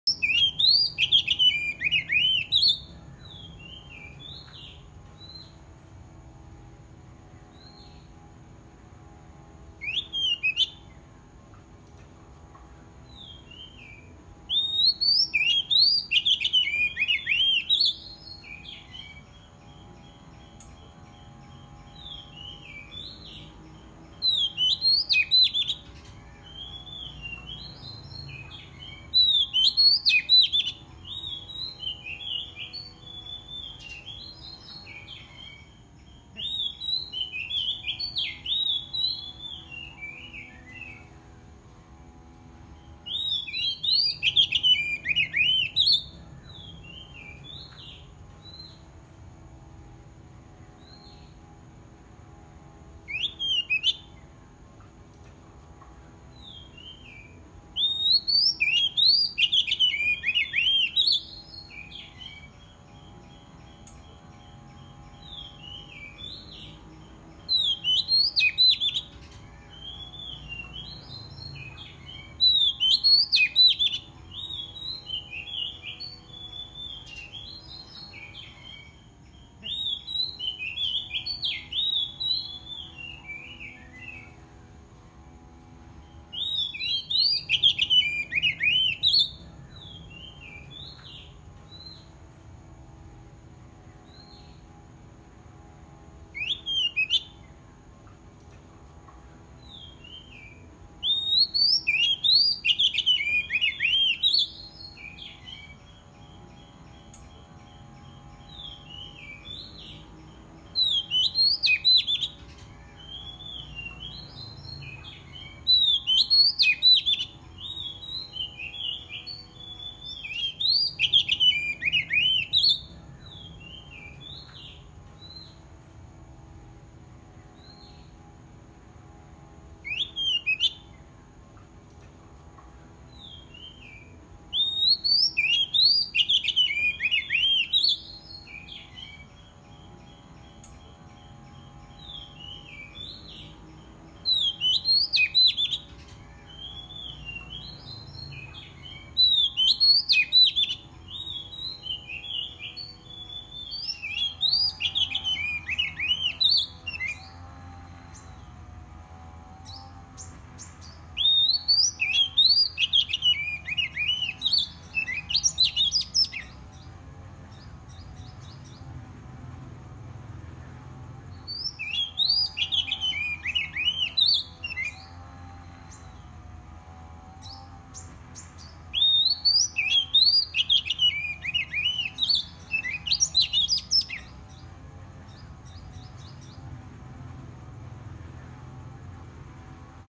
Denak Murai Cacing Galak - Kacer Gacor Cari Lawan - Chòe Than Mái 2021
Suara Murai cacing @ murai kampung galak biasa ajar denak.. tapau banyak... Kacer GACOR pancingan untuk nyari lawan. Suara ampuh Memanggil lawan. Chòe Than Mái